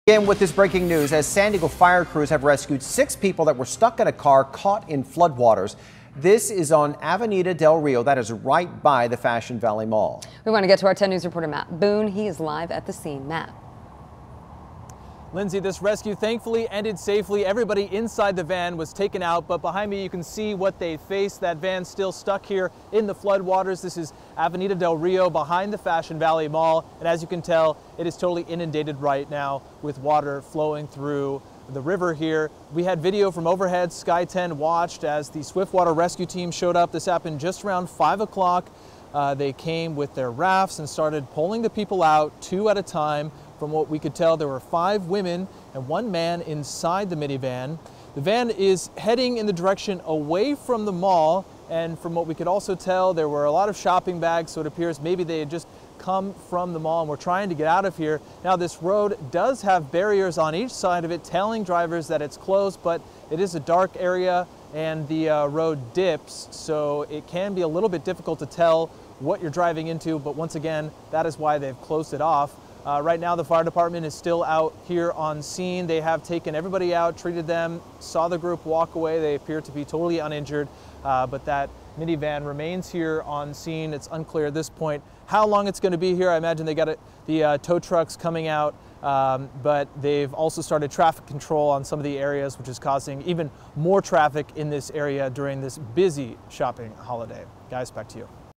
Swift water rescue team makes save near mall
The minivan drove down a flooded street.